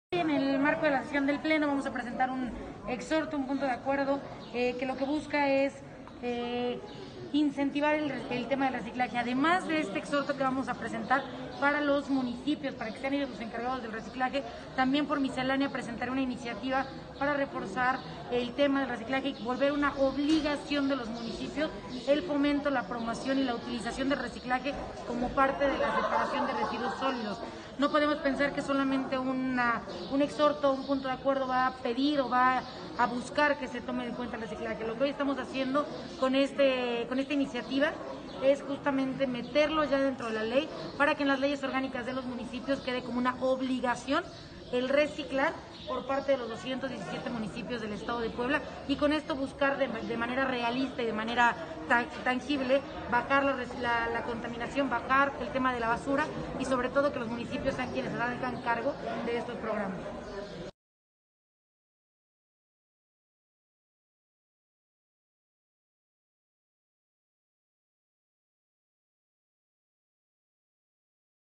Nora Merino pide investigación contra titular de la ASE por registros irregulares
La presidenta del H. Congreso del Estado de Puebla , Nora Escamilla , anuncia una iniciativa para que, por ley, los ayuntamientos impulsen la creación de centros de reciclaje.